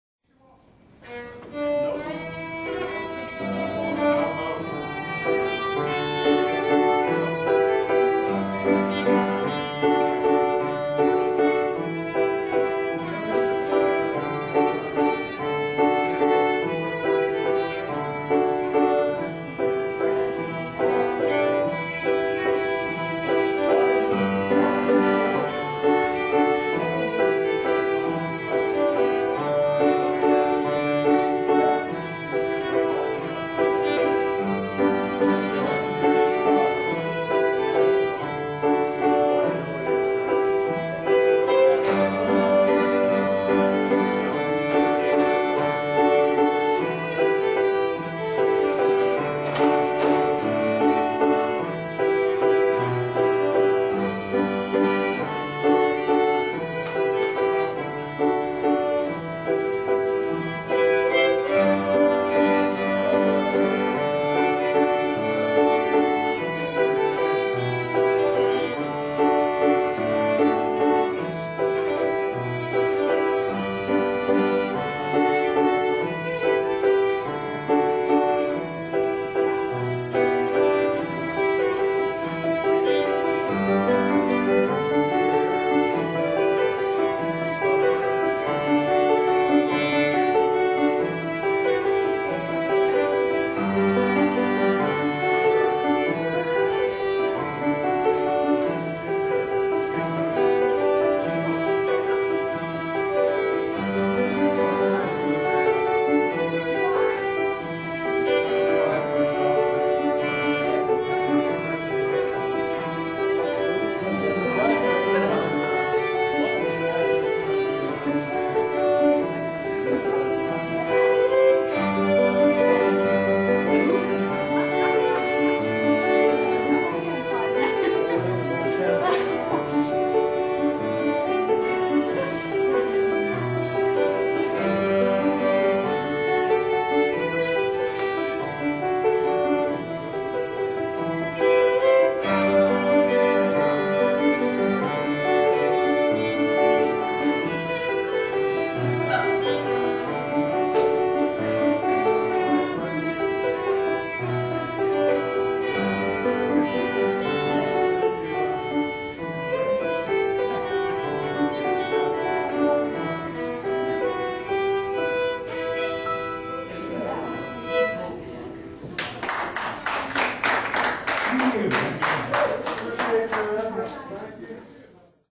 2007-08-18 - 11 - Track 11
Contra dance, performed to song_title recorded live at the United Methodist Centenary Church.